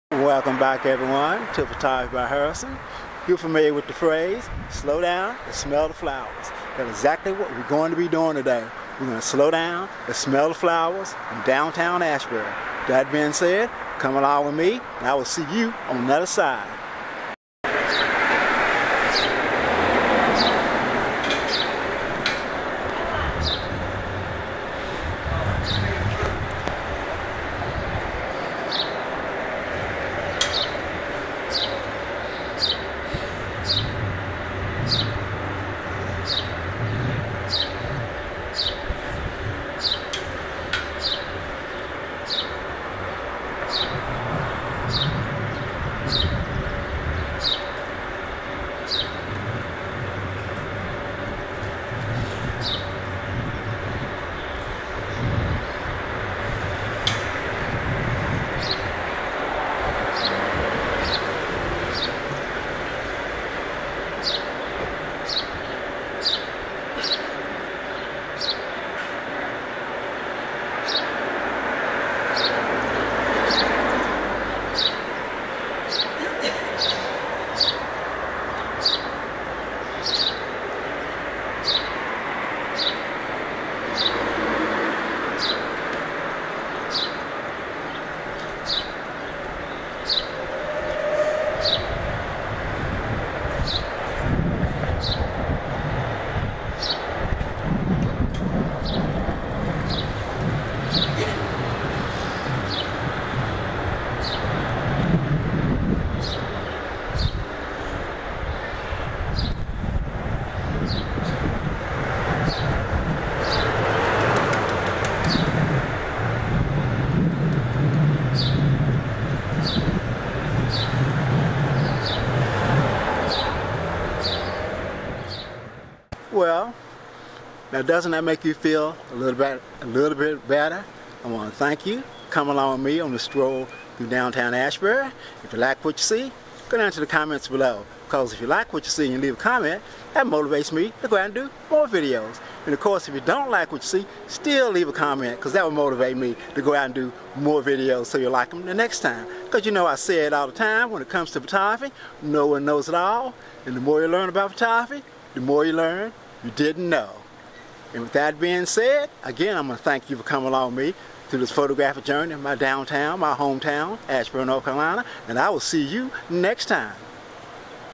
A quick scroll downtown to enjoy the flowers and the sounds of the city, with the FZ1000ii
Come along with me as I take you on a lazy walk around downtown Asheboro to enjoy the flowers and listen to the sounds of the city.
The camera used Panasonic FZ1000ii shutter speed 400, ISO 125, F 5.6 manual, and autofocus.